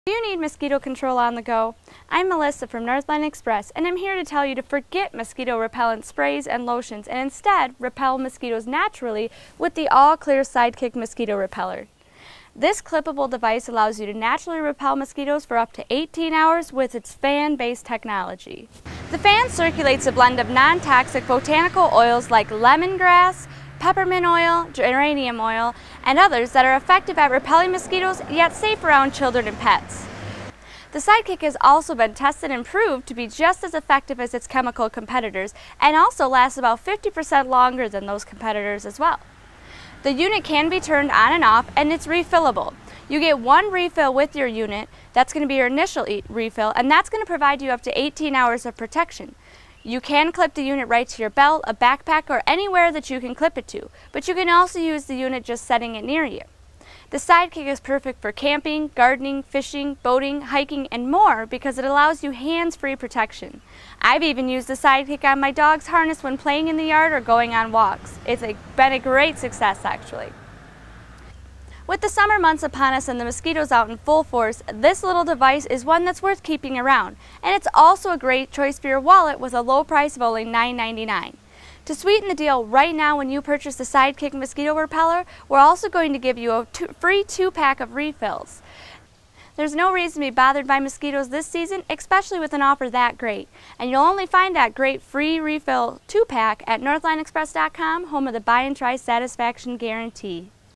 No More Greasy Bug Sprays with the AllClear SideKick Mosquito Repeller
to view our large product inventory at NorthlineExpress.com.

Video Highlights:
0:12 About the AllClear SideKick Repeller
0:31 Tested and Proven Effective
0:43 18 hrs. of Protection Included w/ purchase
0:49 Where and When to clip the unit
1:12 Promotion Details

Do you need mosquito control on the go? This clip-able device allows you to naturally repel mosquitoes for up to 18 hours with is fan-based technology. The fan circulates a blend of non-toxic botanical oils like lemongrass, geranium oil, and peppermint oil that are effective at repelling mosquitoes and safe around children and pets. You can clip the unit right to your belt, a back pack, or anywhere that you can clip it to. You can also use the unit just setting it near you.

to purchase your very own AllClear SideKick Mosquito Repeller from NorthlineExpress.com today!